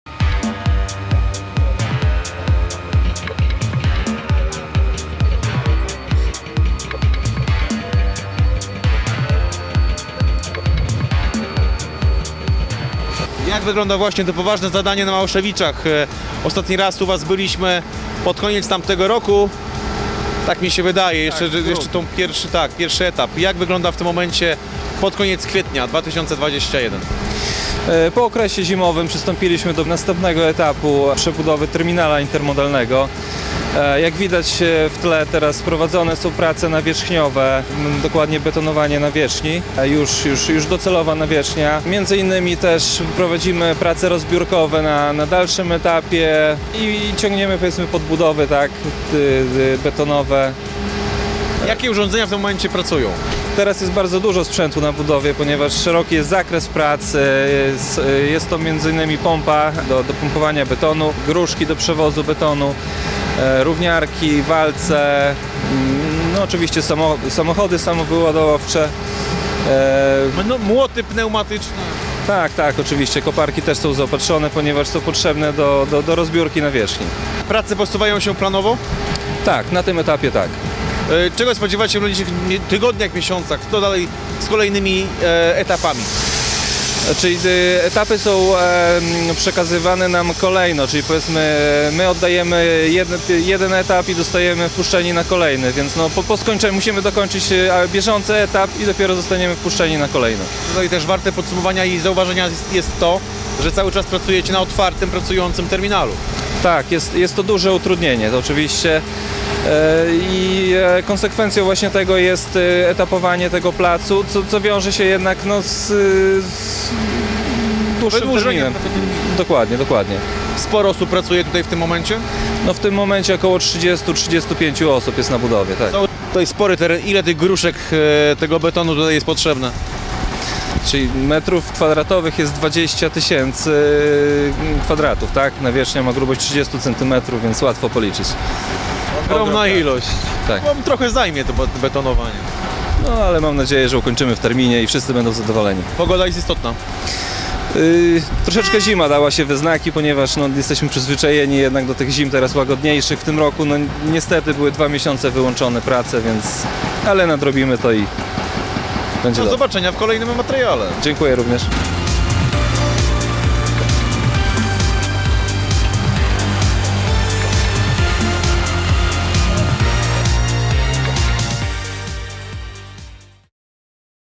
Kolejny etap przebudowy terminala intermodalnego PKP Cargo w Małaszewiczach
Firma Transbet rozpoczęła kolejny etap przebudowy terminala intermodalnego PKP Cargo w Małaszewiczach.